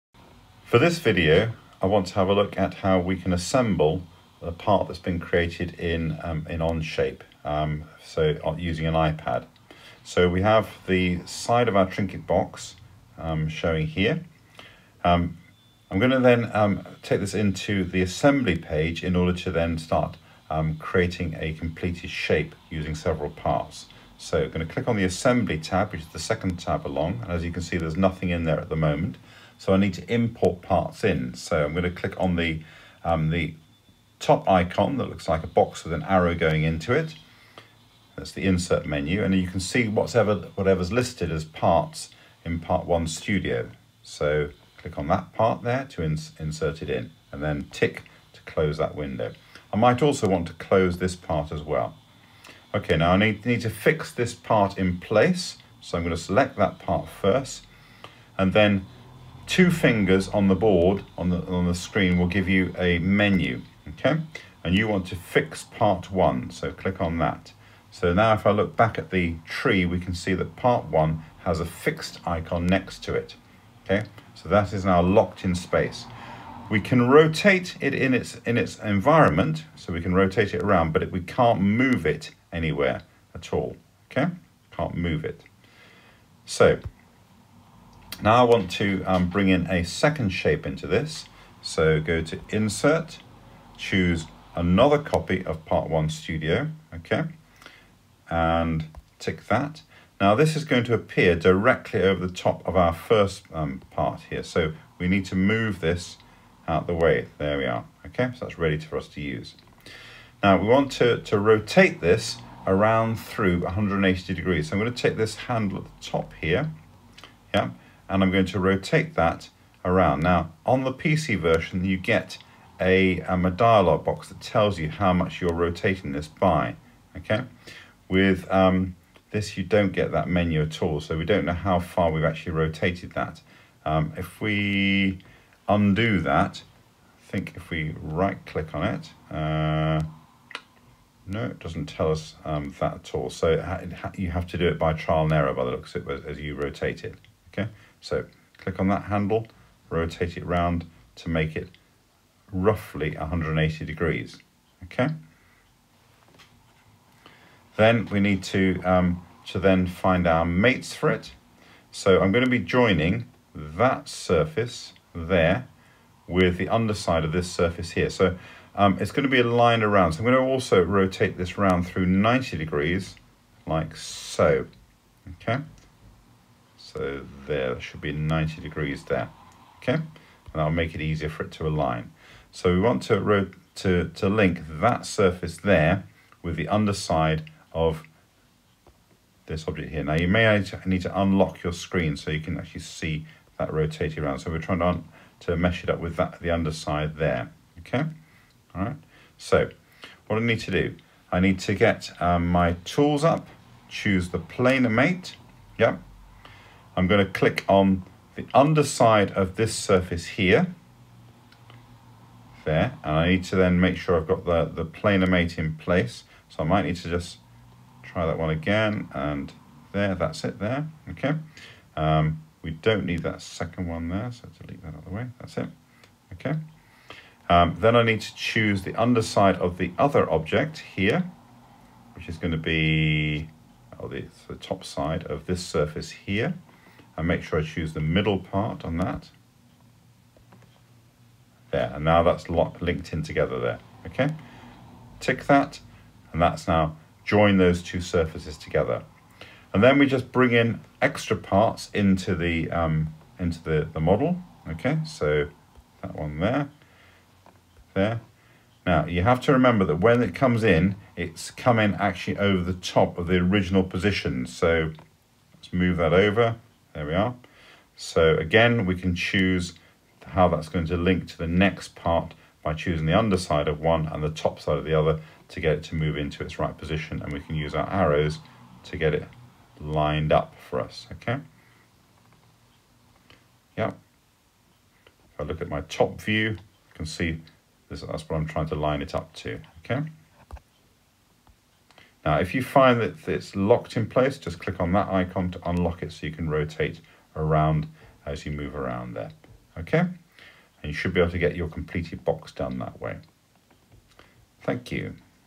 Trinket box assembly on ipad 2nd version
This is a slightly improved version of how to assemble parts in Onshape on an iPad